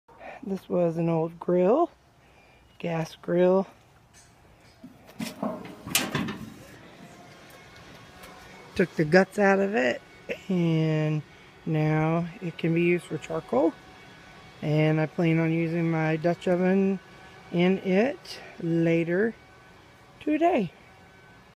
Gas Grill turned into Charcoal Grill/Outdoor Cooking Table/Camp Dutch Oven cooking
Removed inside parts of my old gas grill to use as a charcoal grill/cast iron cooking table/Camp Dutch oven charcoal grill thingie

I couldn't get the video of how I did it to upload. I removed the dials, inner bucket pieces, and lined it with aluminum foil. There are holes in the bottom, and the Dutch oven legs pop through them, so I have to be careful how I set the oven when it's inside there.